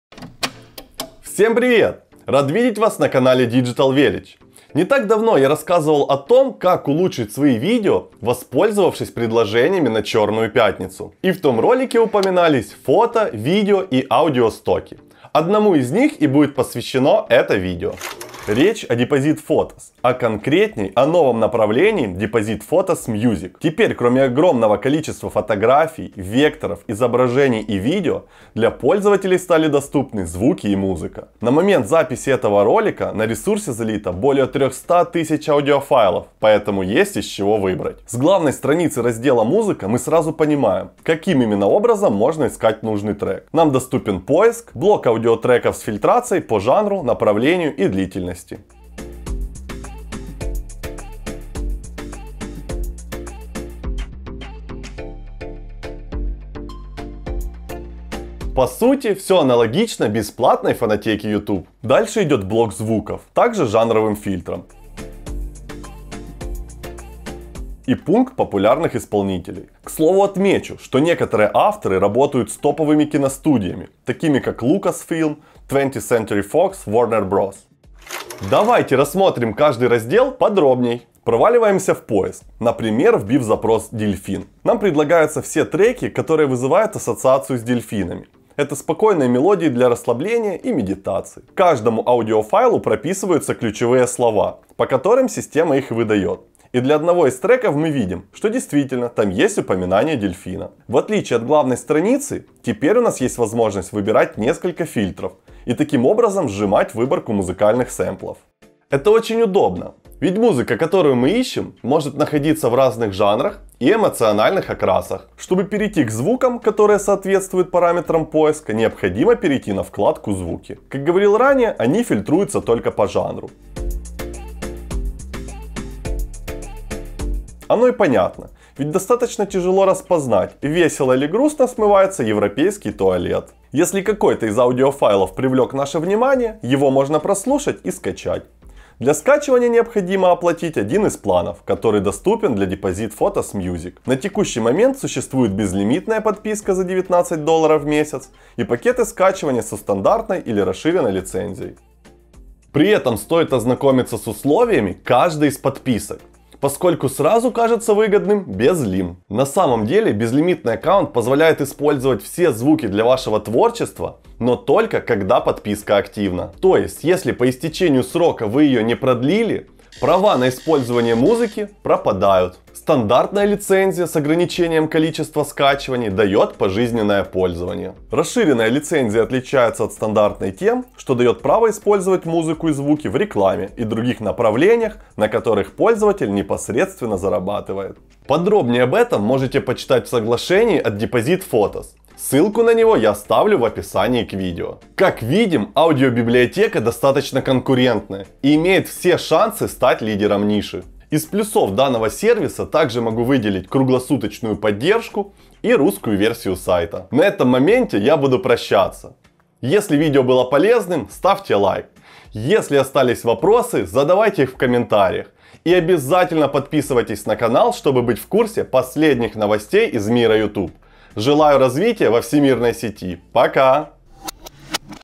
Depositphotos Music - Музыка и Звуки для YouTube видео
Обзор нового сервиса от Depositphotos, с большим количеством музыки и звуковых эффектов.

Известный большому количеству дизайнеров и креаторов видео- и фотобанк Депозитфотос, запустил новый раздел с роялти-фри стоковыми аудио, музыкой и звуками. Подробные условия каждой из лицензии можно прочитать в условиях соглашений
Как и большинство других сервисов по продаже звуков и музыки, есть удобная система поиска нужных мелодий. Так вы можете использовать фильтры по ключевым словам, жанрам, направлениям, длительности, исполнителю. Поэтому, если вы переросли Фонотеку Ютуб, можете попробовать платный вариант. Если у вас появятся вопросы по Depositphotos Music - можете задавать их в поддержку сервиса. Тем более, что она круглосуточная и русскоязычная.